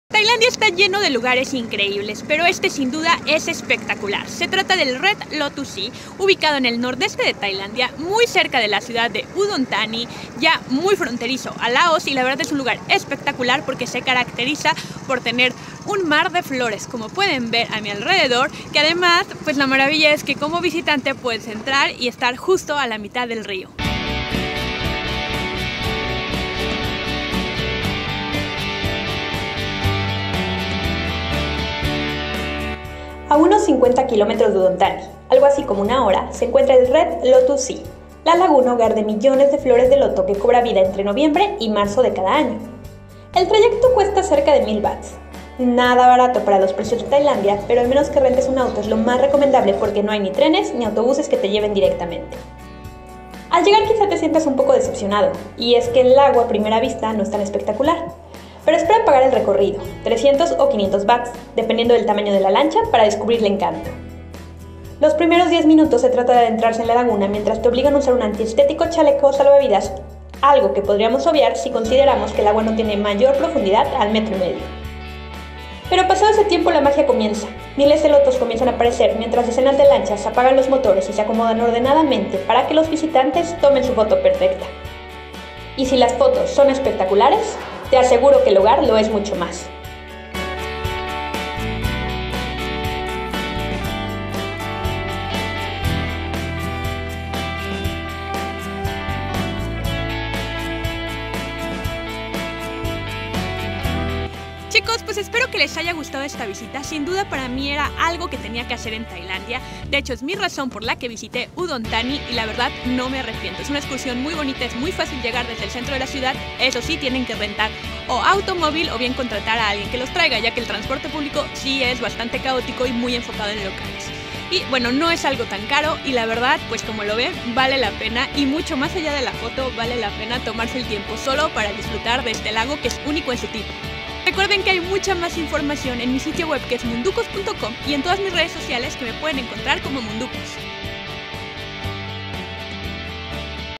Red Lotus Sea: mar de flores en Tailandia
Les presento el Red Lotus Sea, el mar de flores de Tailandia ubicado en Udon Thani. Todos los detalles